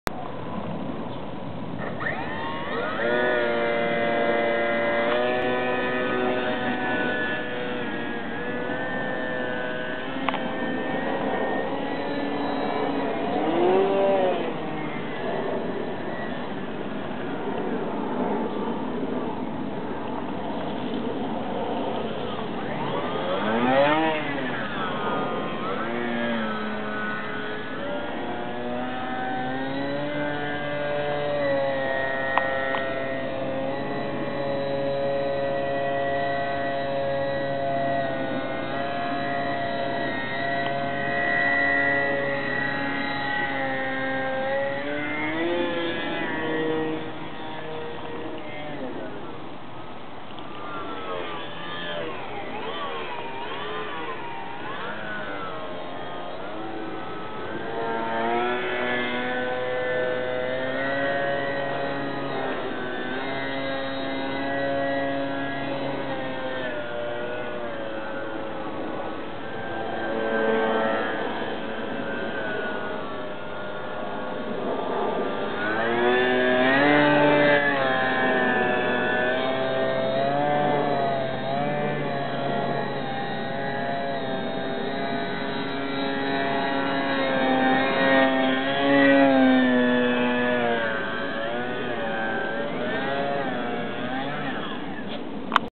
Cartoon 3D GeeBee.MOV
I was bored,about an hour to throw this together with alot of junk I had sitting around. flies better than I expected,not bad for next to nothing in cost.